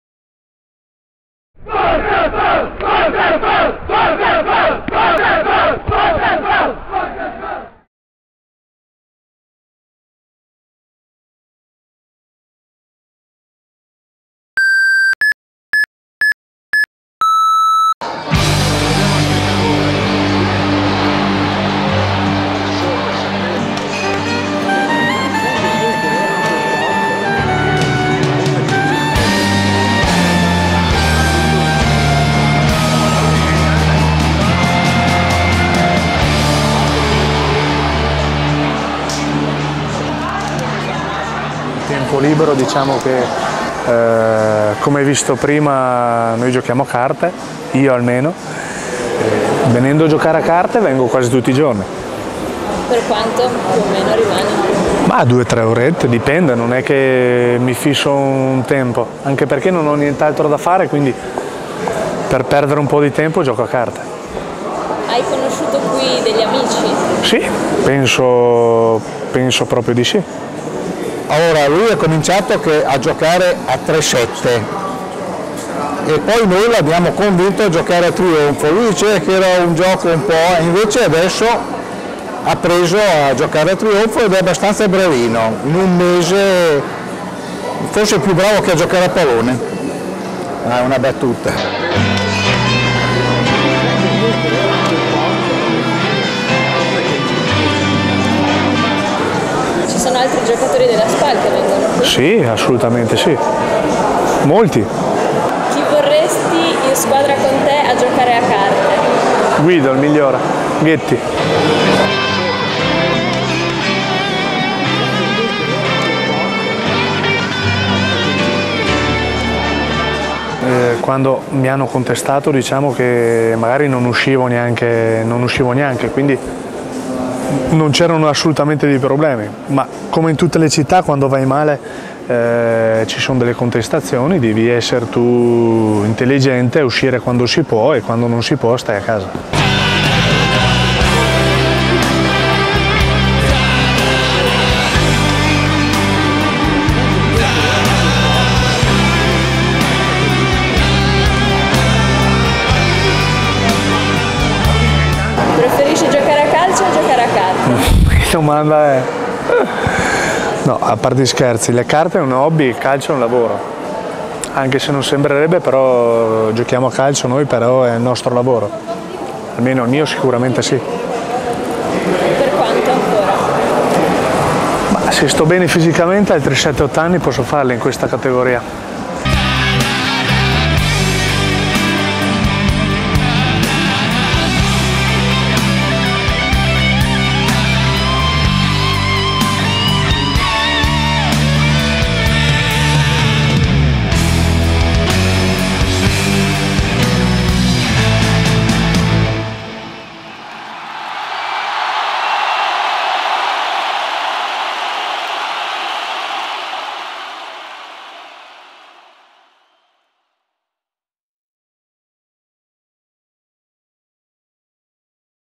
INTERVISTA A MARCO ZAMBONI 3
- Una giornata con Marco Zamboni, capitano della Spal.
Terza parte, la sera.Realizzato da Stefania Andreotti. Ferrara, Italia 2009.